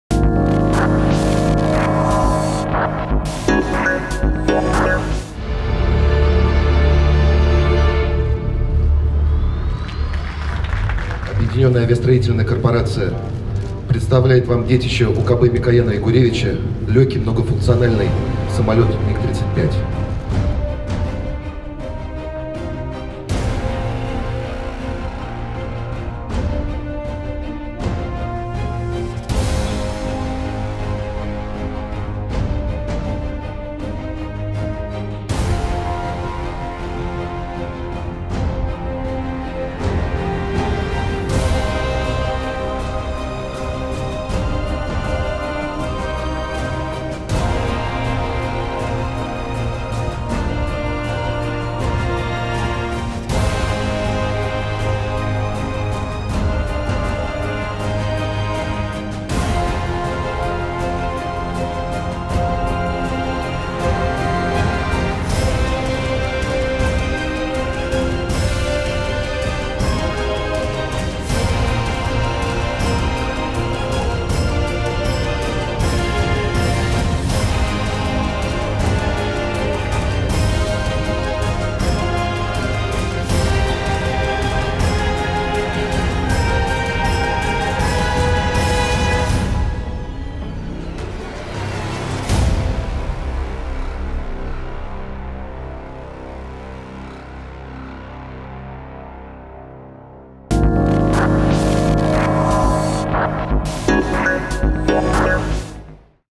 EL NUEVO MiG 35 en 360º Presentación oficial del nuevo caza ruso de Mikoyan
Vídeo Noticia EL NUEVO MiG EN 360° Presentación oficial del nuevo caza ruso